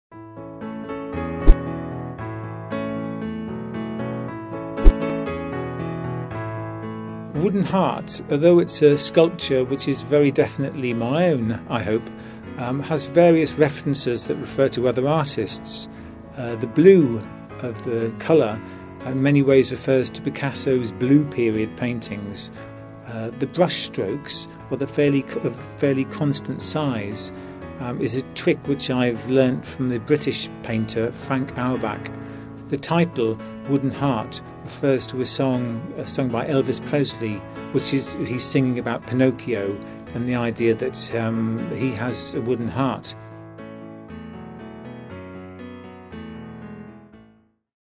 A Guide to Glenn Brown: “Wooden Heart”
Glenn Brown shows us the ins and outs behind some of his favorite paintings and sculptures in his first solo exhibition here at the CAC. Take a look at "Wooden Heart", a large blue sculpture that may have been inspired by Pinocchio.